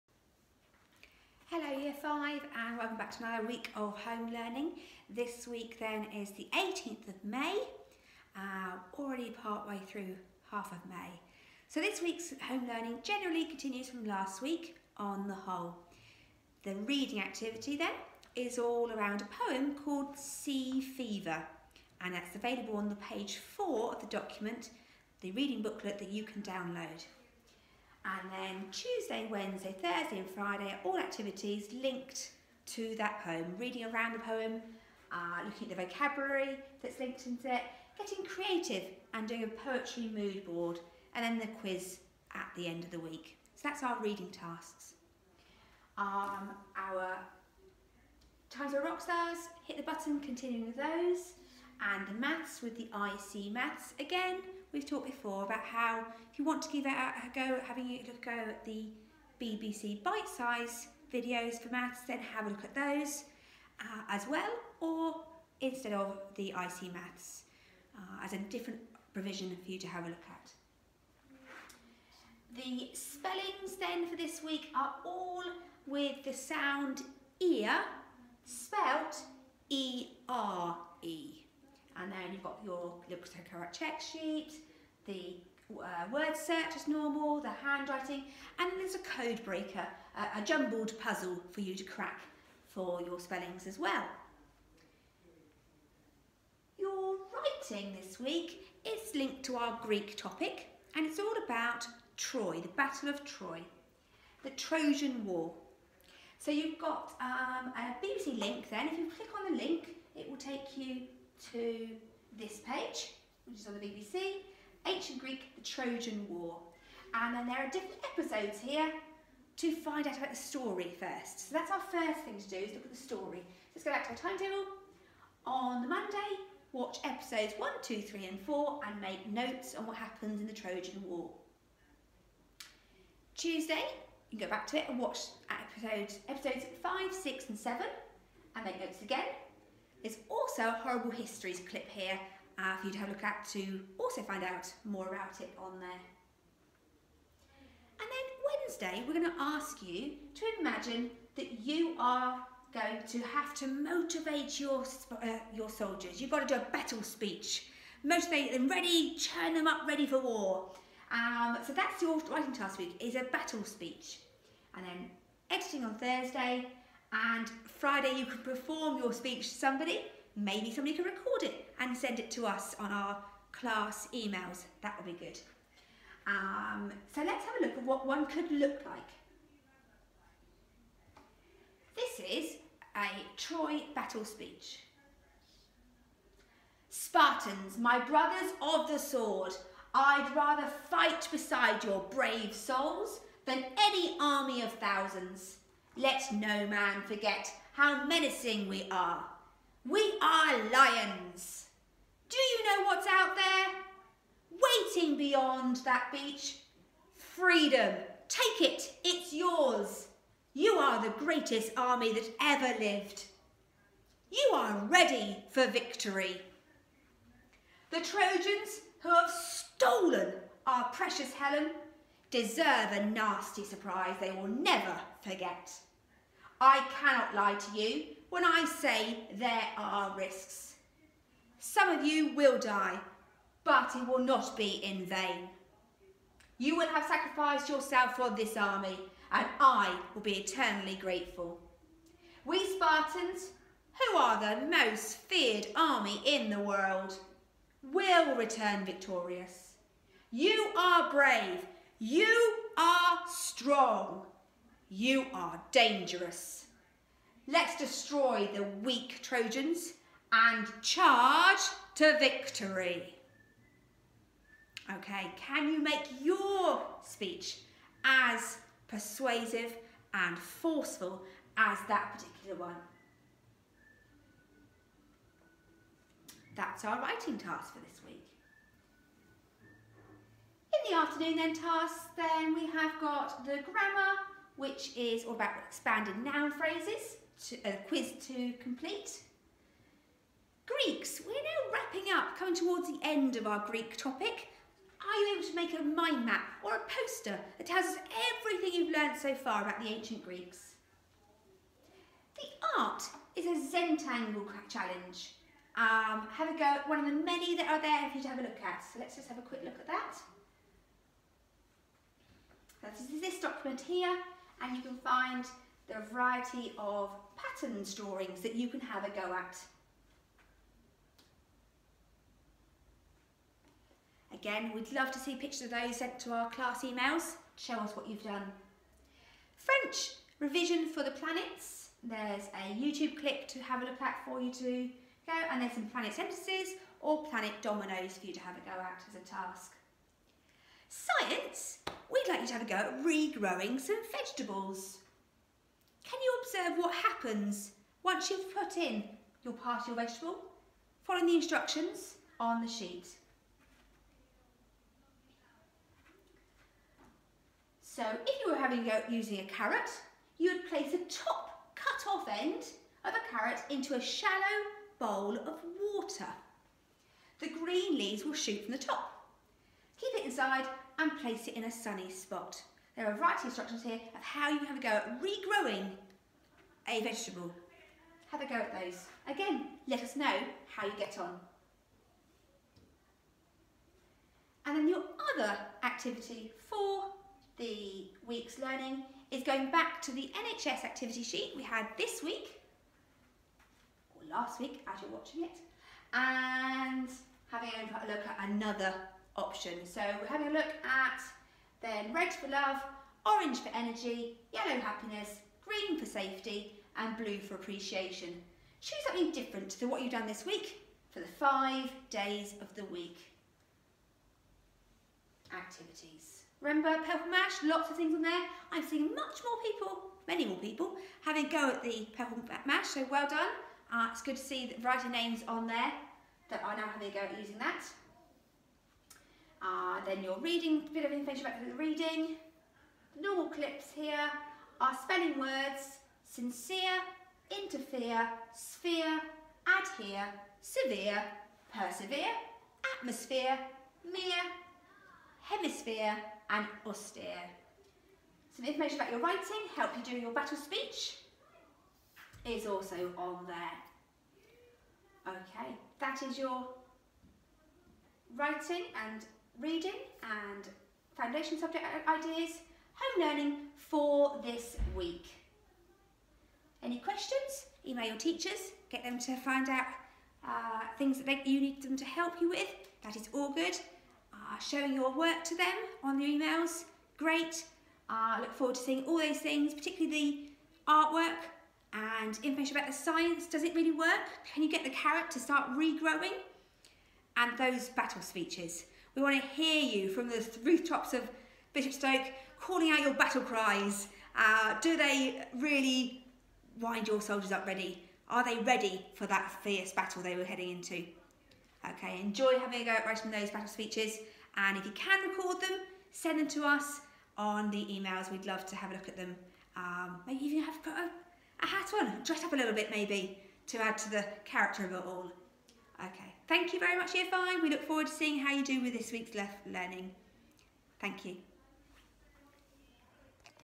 Year 5 home learning 18.5.20
Mrs Smith talks through Year 5 home learning for the week beginning 18th May.